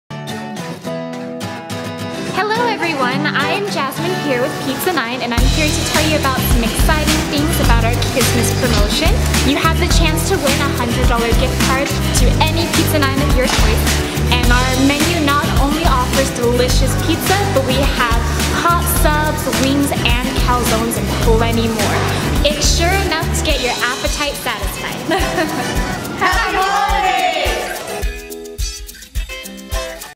Kissmas Day 9 : Pizza 9
Day 9: Whether your'e having a party, a family get together, or are just hungry head into Pizza 9 for all your hunger urges! Listen to Gina Lee Fuentez in the A.M. and DJ Animal in the afternoon for your chance to win a $100 Pizza 9 gift certificate.